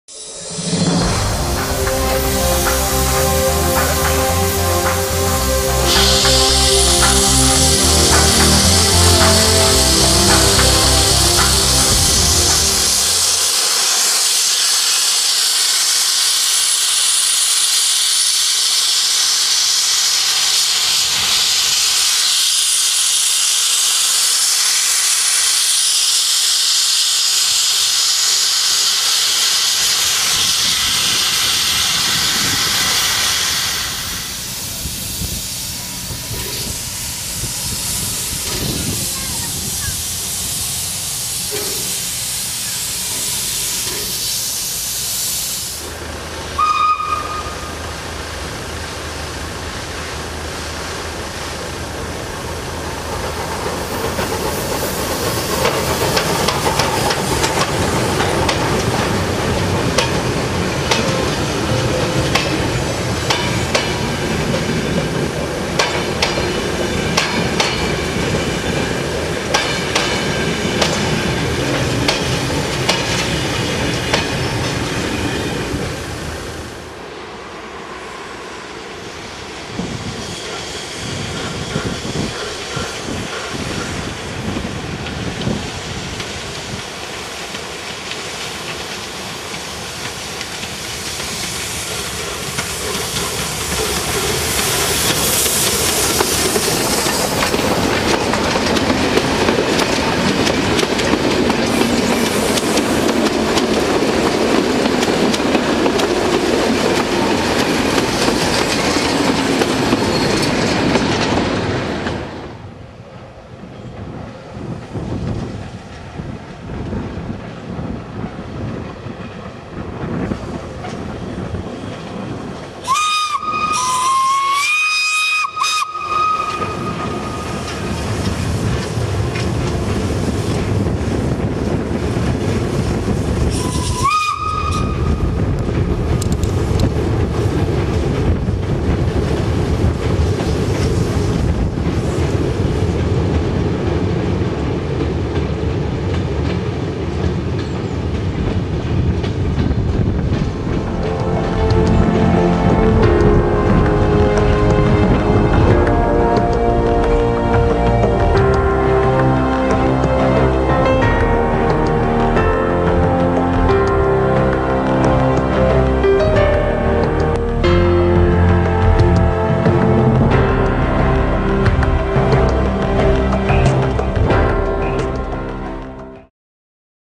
La Vapeur du Trieux
"La Vapeur du Trieux" ist ein Touristenzug, der die Strecke von Paimpol nach Pontrieux, im Tal des Trieux befährt. Normalerweise verkehren hier die Triebwagen des TER Bretagne von Paimpol nach Guingamp. Die Strecke war einst ein Teil eines riesigen Meterspurnetzes, dass zu seiner Blütezeit eine Länge von etwa 425 km hatte. Bereits 1924 wurde die Strecke Guingamp - Paimpol auf ein Dreischienengleis erweitert, 1952 wurde der Schmalspurbetrieb eingestellt.